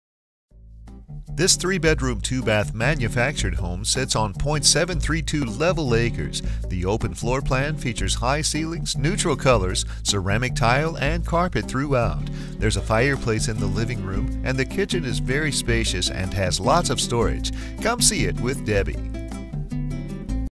Home for sale in Bandera, TX | $122,000
3BR, 2BA manufactured home sits on 0.732 level acres. Open floorplan, high ceilings, neutral colors, ceramic tile &amp; carpet throughout. Fireplace in living room, spacious kitchen with lots of storage. master bedroom has walk-in closet, master bath has jetted tub. Mature trees. Come see it!